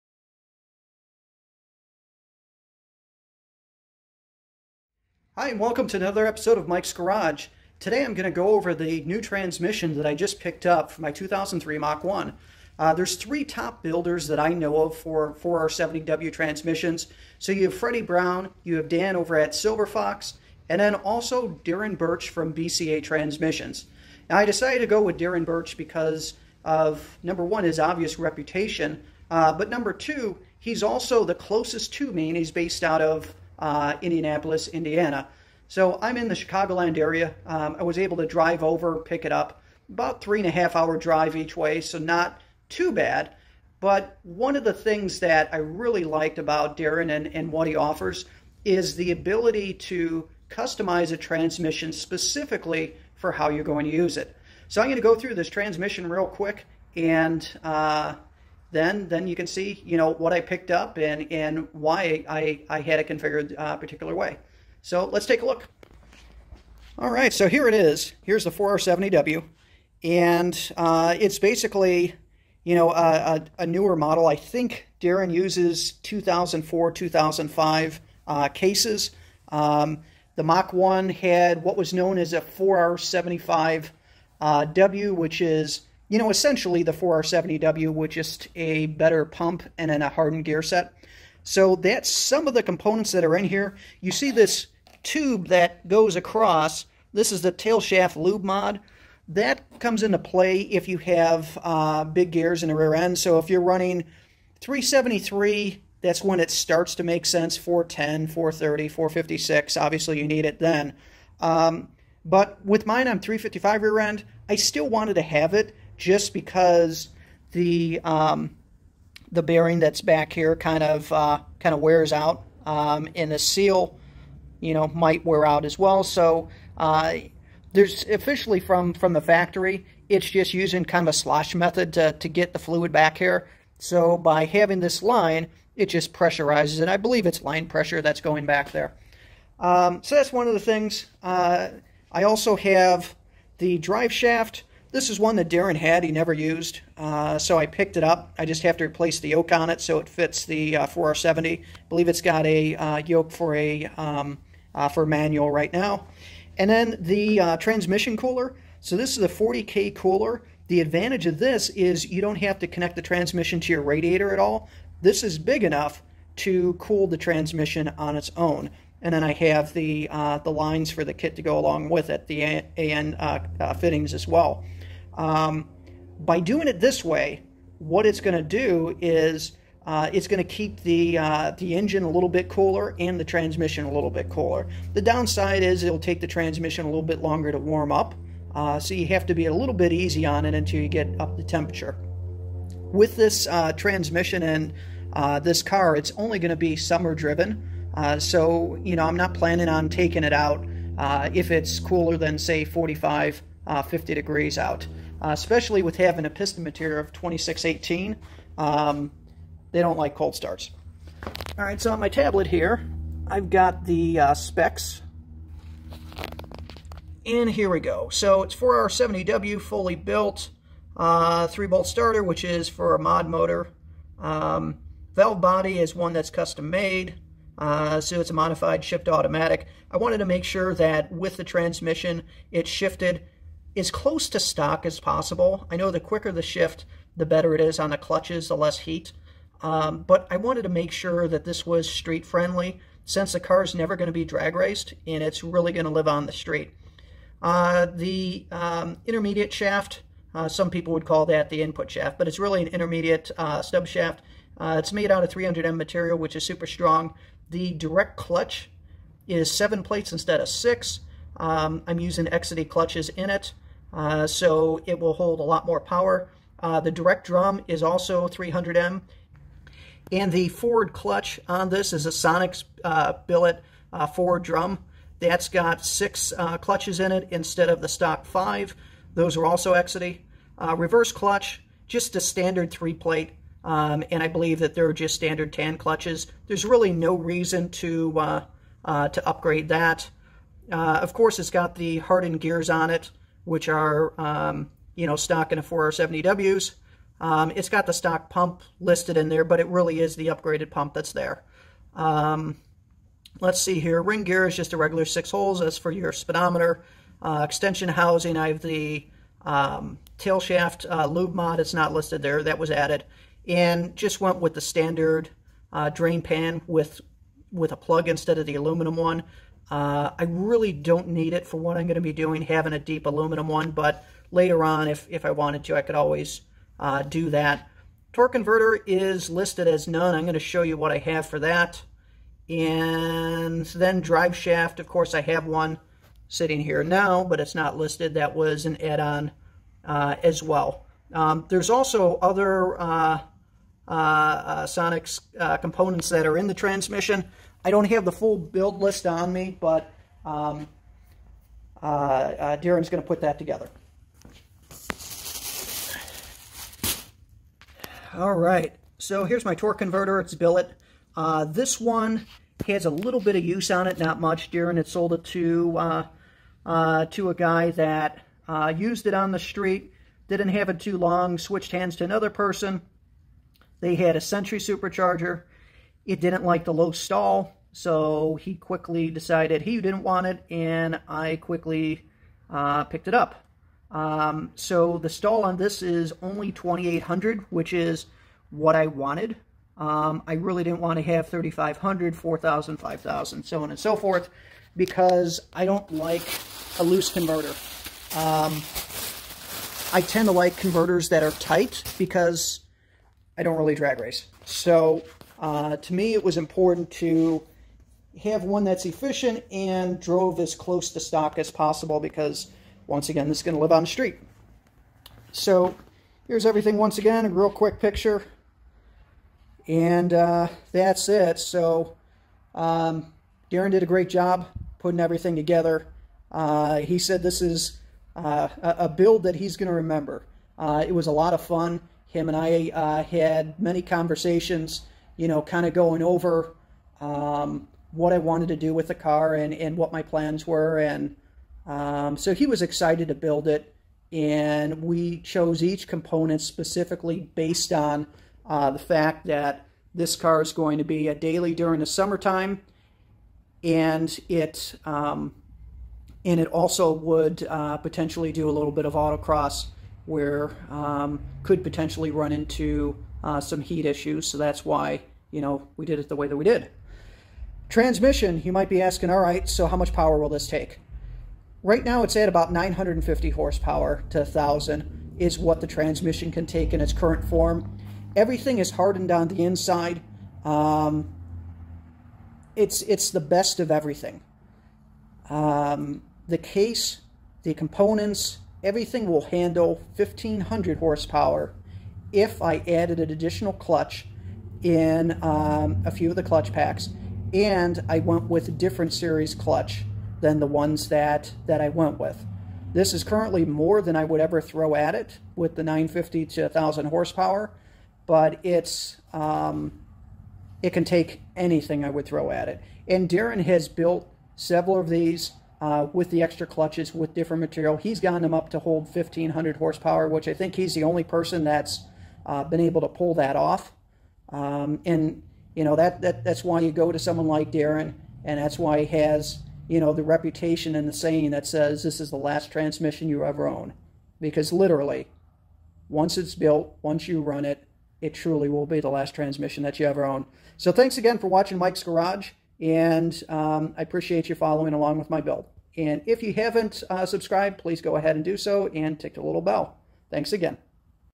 4R70W Review for my 2003 Mach 1 Mustang (Built by Darrin Burch at BCA Transmissions)
4R70W Review for my 2003 Mach 1 Mustang.  This transmission was made by Darrin Burch at BCA transmissions!  4R70W is build for high horsepower drag racing, but also for road racing my Mach 1 Mustang.